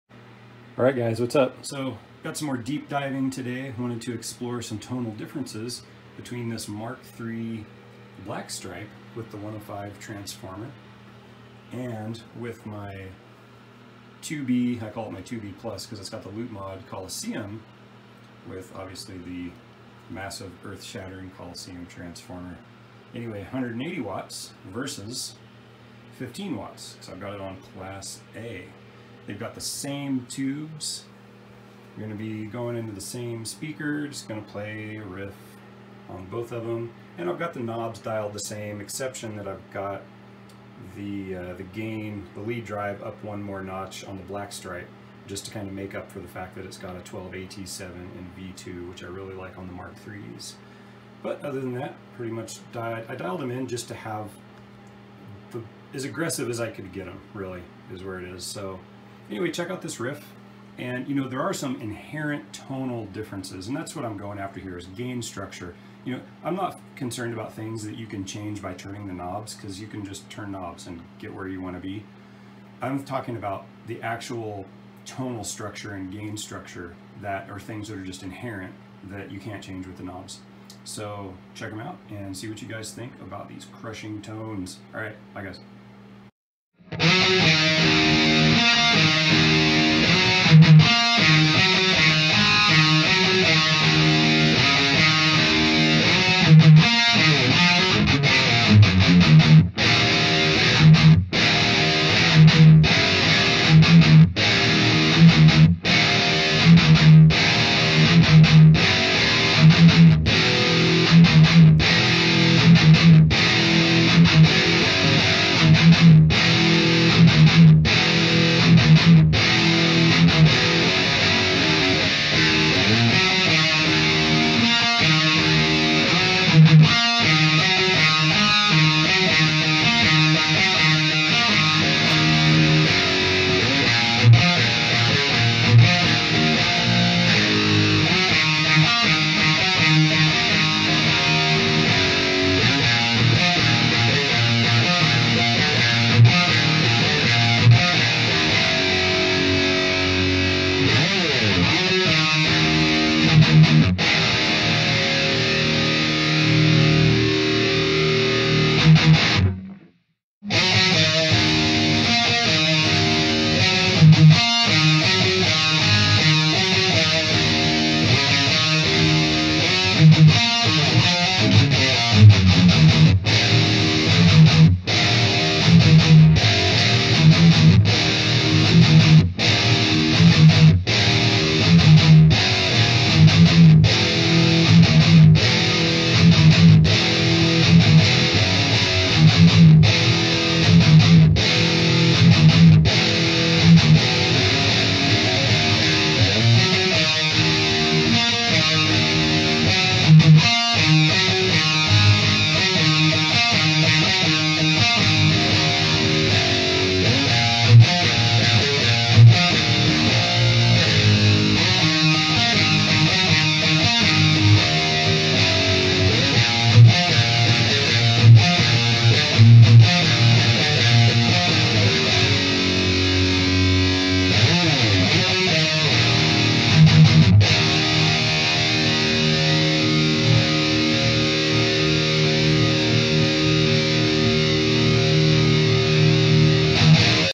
Mesa Boogie Mark III black stripe vs. Mark IIB loop mod
Mark III black stripe 1:26
Mark IIB+ 2:40

This IIB is the only Mark I've ever played that doesn't have that inherent nasally mid character. Is it amp-to-amp variation? Is it the power section? Hard to say. Anyway, what do you think about the difference?